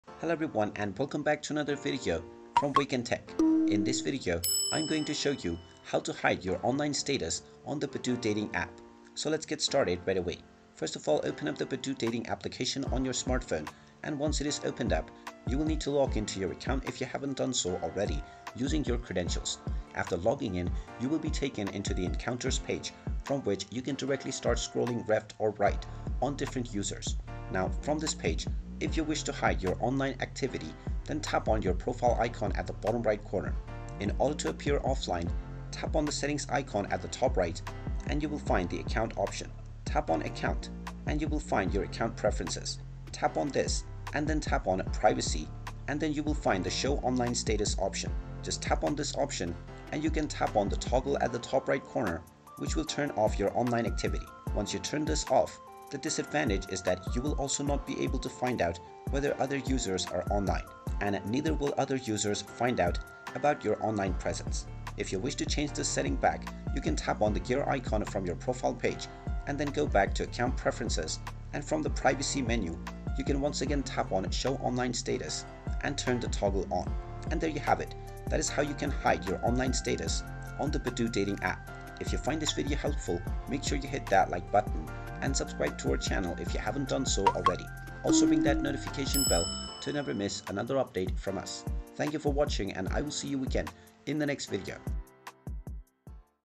How to Hide your Online Status on Badoo Dating App 2024?
Want to maintain a bit of privacy while using the Badoo dating app? Learn how to hide your online status with our simple tutorial! Whether you're looking for some alone time or just want to browse without interruptions, hiding your online status can give you the peace of mind you need.